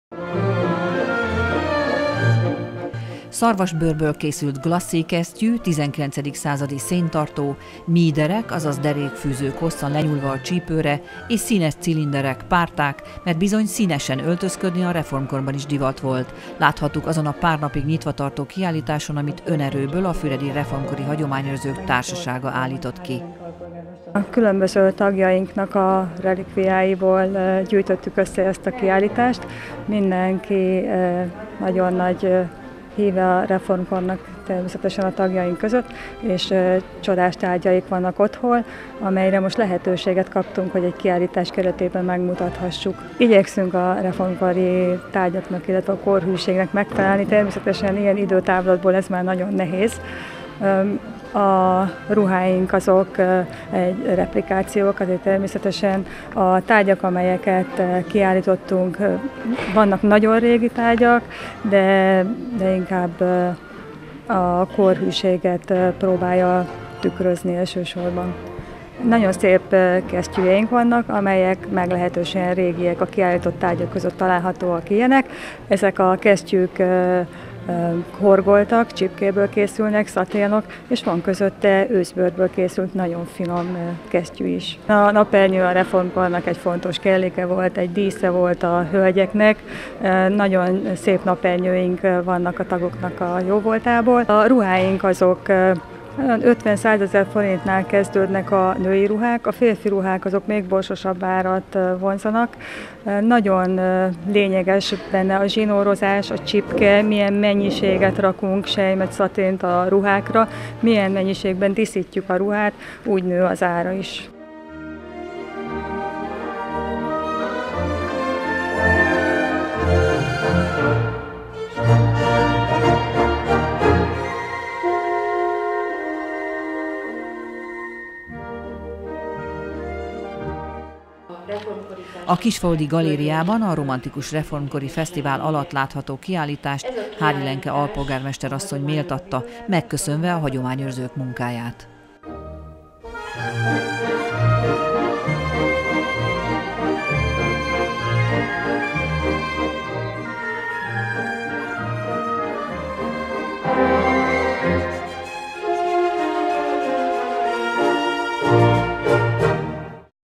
A tárgyak is meséltek
Sokkal színesebben öltöztek a reformkorban, mint azt ma gondolnánk. A férfiak is színes, piros, sárga, zöld öltözékeket hordtak, a nők pedig kedvelték a kockás ruhákat is. A Romantikus Reformkor Fesztivál alkalmi kiállításán előkerültek olyan tárgyak, amiket ma már alig ismerünk, de a hagyományőrző egyesület tagjai jóvoltából, pár napig megcsodálhattuk.